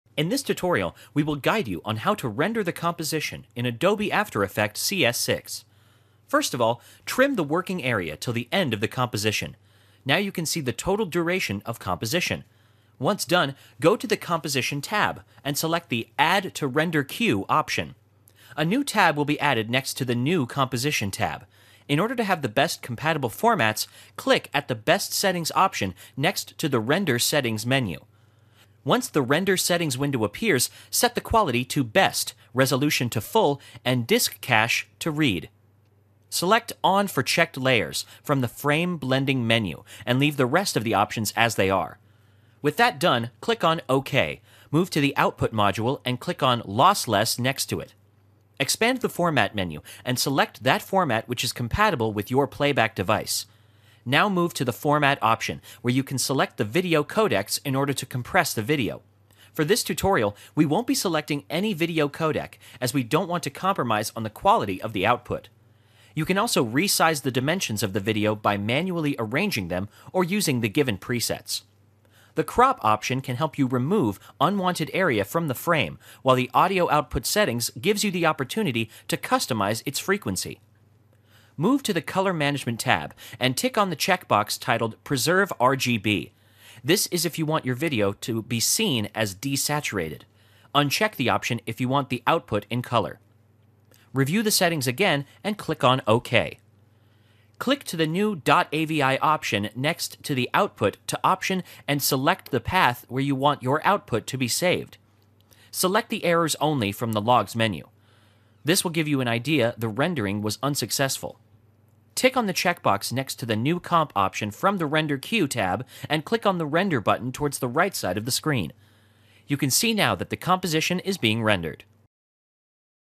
How to Render in Adobe After Effects CS6
Follow this step by step guide to learn how to render in Adobe After Effects and make the most of this useful compositing tool.

Adobe After Effects gives you a long chain of formats to save your output in. This is important so that you get the most compatible extension that really fits well with your media. Since it is a compositing tool, it also produces frame to frame animation formats such as PNG, PSD, TIFF sequences which are applicable for storyboarding. But before you get to choose a format for your output, it's important to know how to render in After Effects.
In this tutorial, we will guide you on how to render in After Effects CS6

Step # 1 -- Add to Render Queue Option

In order to render in after effects, start with trimming the working area till the end of the composition. Now, you can see the total duration of composition. Once done, go to the composition tab and select the "add to render queue" option.

Step # 2 -- Best Settings Option

A new tab will be added next to the "new" composition tab. In order to have the best compatible formats, click at the "best settings" option next to the render settings menu.

Step # 3 -- Adjust the Render Settings

Once the render settings window appears, set the quality to best, resolution to full and disk cache to read. Change these settings to proceed further with our render procedure in After Effects.
Select "on for checked layers" from the frame blending menu and leave the rest of the options as they are.

Step # 4 -- Adjust Output Module Settings

With that done, click on Ok. Move to the output module and click on lossless next to it.
Expand the format menu and select the format which is compatible with your playback device.
Now move to the format option where you can select the video codec in order to compress the video. For this tutorial, we won't be selecting any video codec as we don't want to compromise on the quality of the output.
You can also resize the dimensions of the video by manually arranging them or using the given presets.
The crop option can help you to remove unwanted area from the frame while the audio output settings give you the opportunity to customize its frequency.

Step # 5-- Change settings for Color Management

Move to the color management tab and tick on the checkbox titled "Preserve RGB". This is if you want your video to be seen as de-saturated. Uncheck the option f you want the output in color. Review the settings again and click on ok.

Step # 6 -- Render Composition

Click to the new.avi option next to the output to option and select the path where you want your output to be saved.
Select the errors only from the logs menu. This will give you an idea if the rendering was unsuccessful.
Tick on the checkbox next to the new comp option from the render queue tab and then click on the "render" button towards the right hand side of the screen. You will see that the composition has started to render.